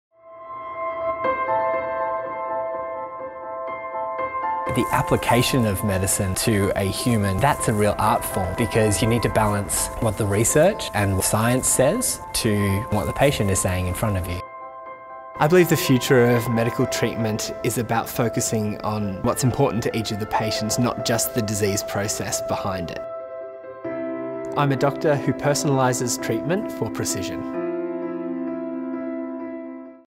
James McGann: a doctor who personalises for precision
"I believe the future of medical treatment is about focusing on the patients and what's important to each of the patients - not just the disease. The Melbourne Model allowed me to develop my thinking outside of the medical setting. You have people who need your help and your support. Being able to sympathise with them and use thought processes to evaluate what’s important to this patient is an invaluable tool."